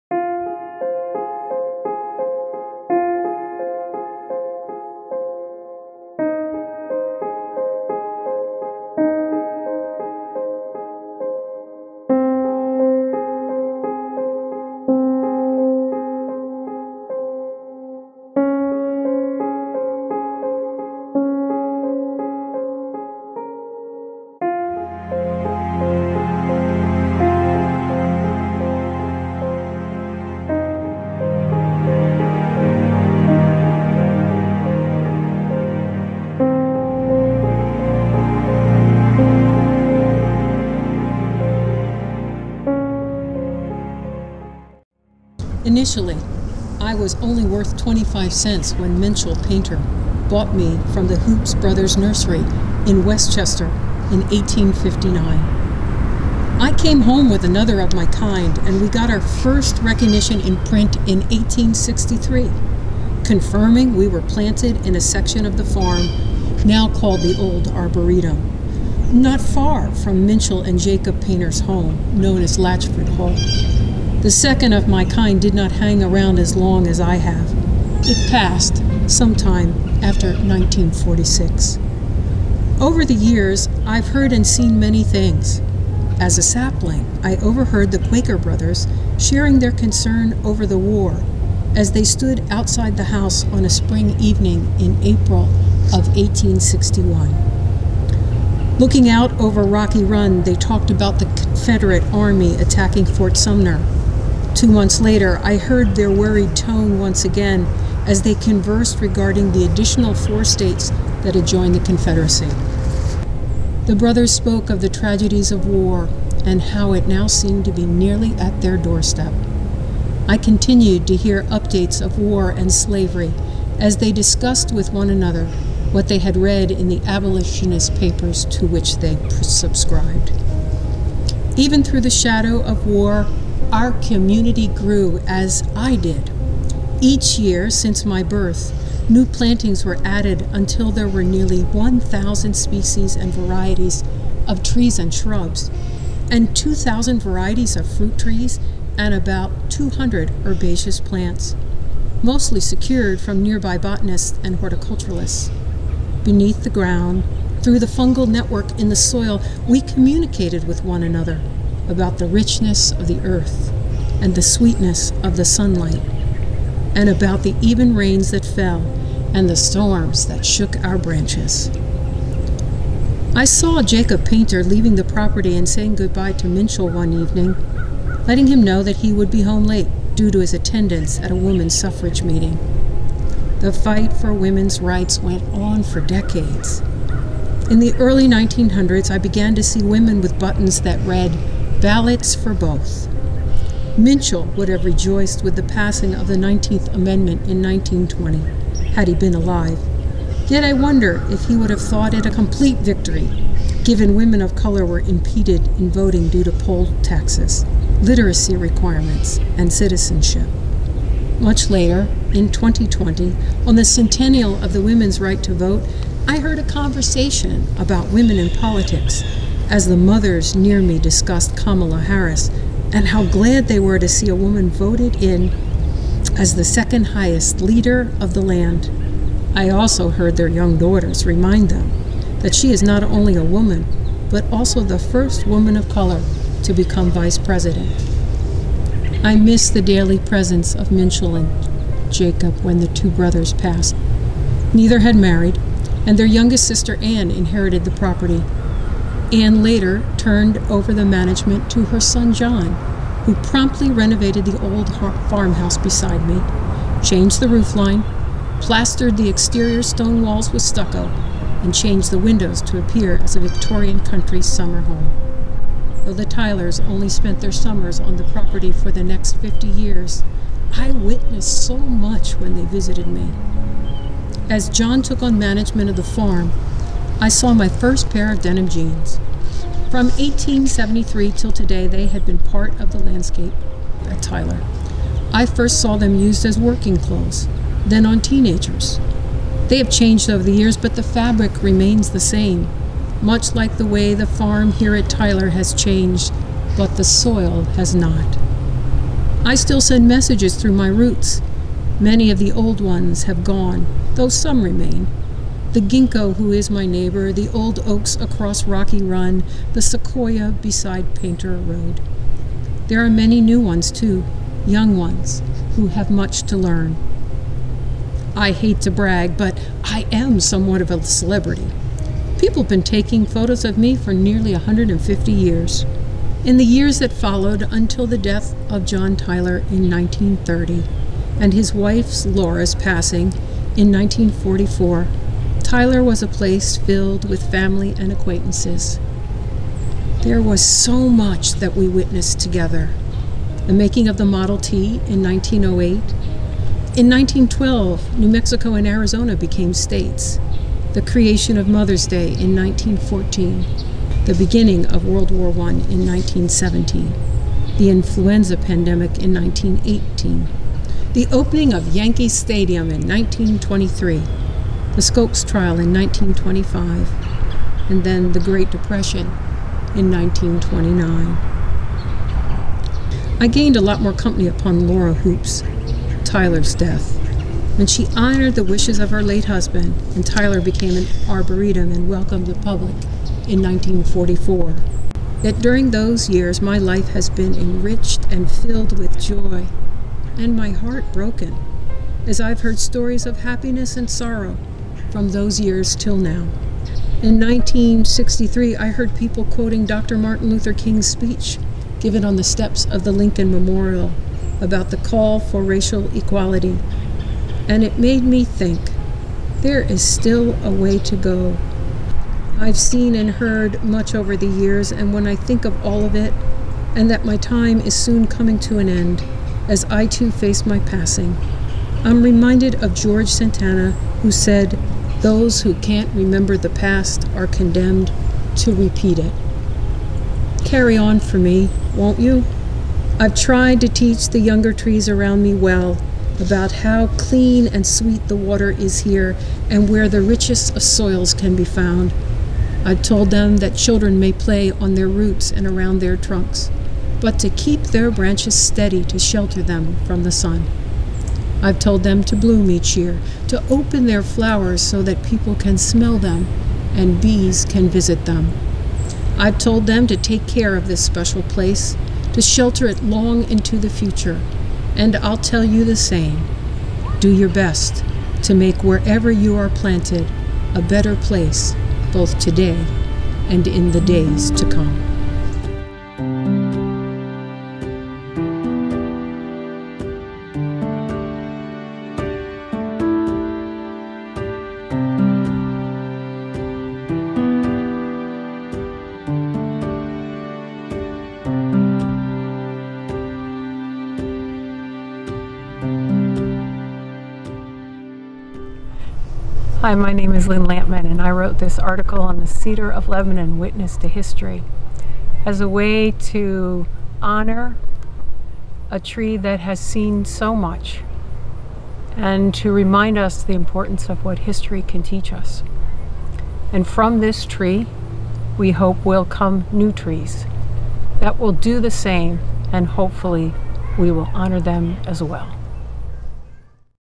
Witness to History Cedar of Lebanon
The death of our Cedar of Lebanon is the physical end, but not the movement it has started.  The Cedar along with the ginkgo, maples and magnolia trees planted on a farm in Pennsylvania by two brothers represent a new approach to seeing the world through nature studies. If we learn one thing, it’s that planting a tree is not for oneself, but for the many generations to come.  This video is a tribute to our beloved tree and the history it has witnessed.